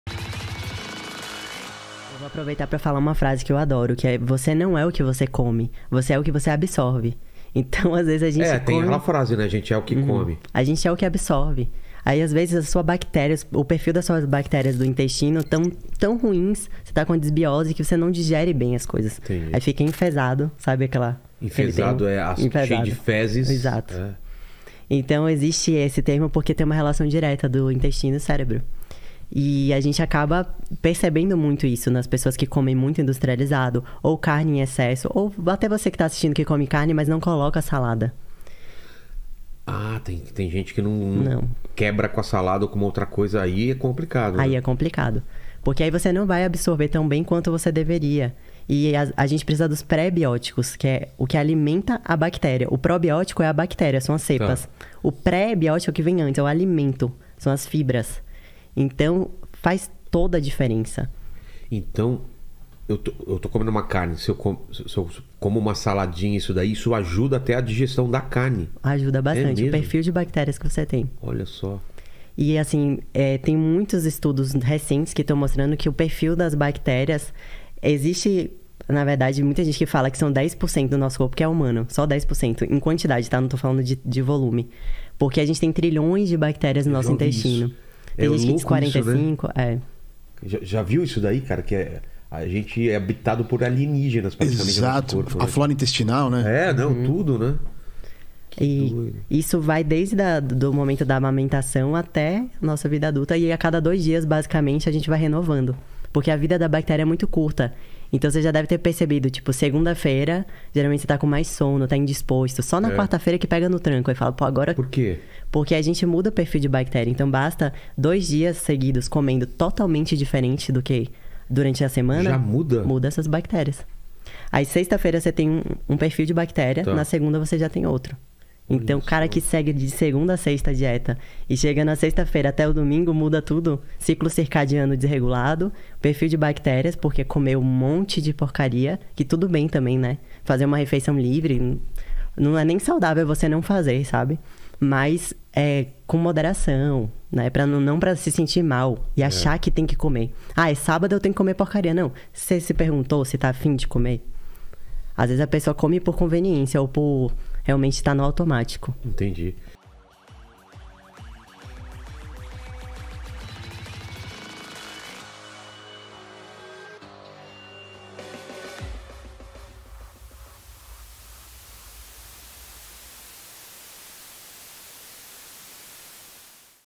A IMPORTÂNCIA DA ALIMENTAÇÃO CORRETA - MARIANA RIOS (VEG CHEF BRASIL)
MARIANA RIOS é nutricionista especializada em nutrição comportamental. Ela é a idealizadora do reality show Veg Chef Brasil, onde mostra que é possível fazer comida sem carne. Já o Vilela só é vegano quando se trata de chocolate.

CAIXA POSTAL
Caixa Postal: 81969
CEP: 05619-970

EDIÇÃO:  Isabelly, Rafael e Yasmin
PRODUÇÃO: Rogério Vilela
PESQUISA E DECUPAGEM: Fabio Mantoanelli